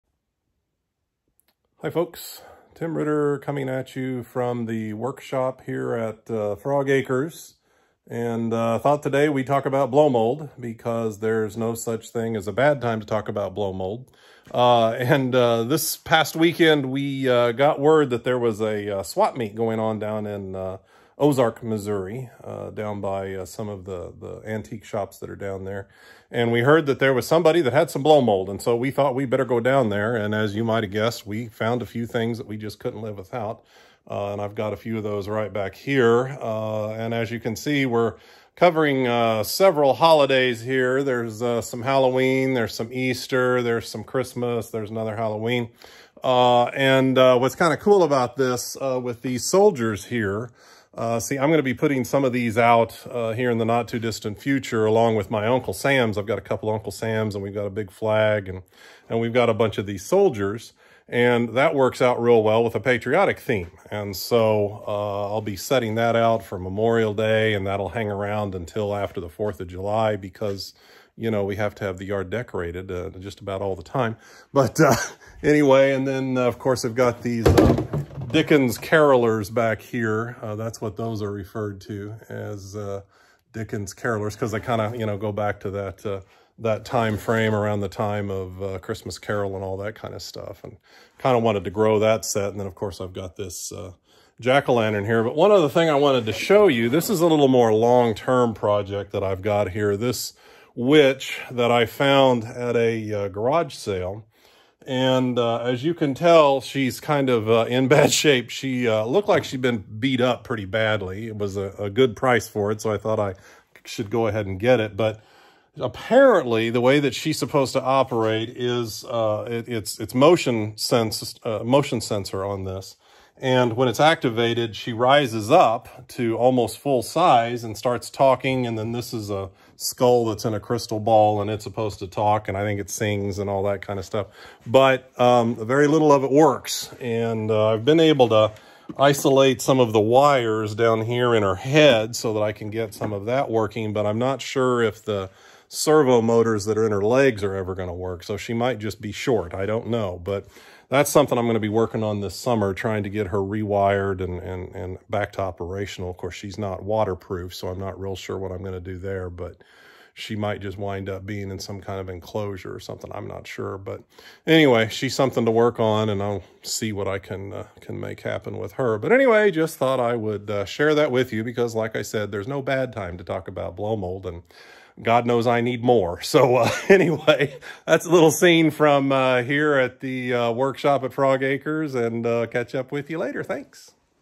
Blowmold Discussion... Because There's No Wrong Time to Talk About Blowmold!
This clip was filmed in the workshop at Frog Acres after we had scored several at an outdoor swap meet. Filmed in July 2022, this was never posted on Facebook. Just a little fun and silliness.